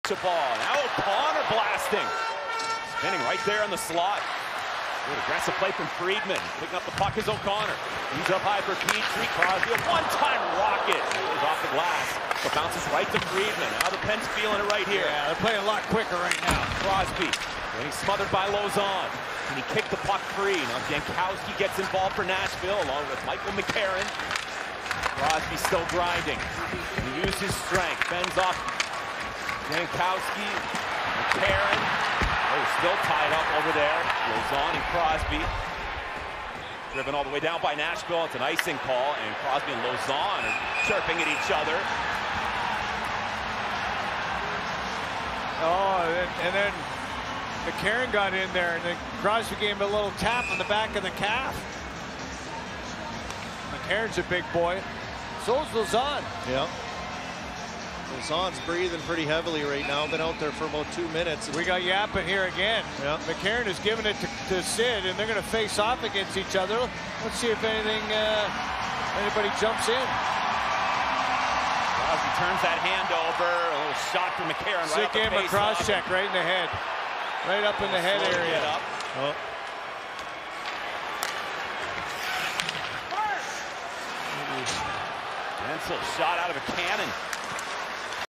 Crosby and McCarron feud
clip from Pittsburgh Penguins vs Nashville Predators on March 30th, 2023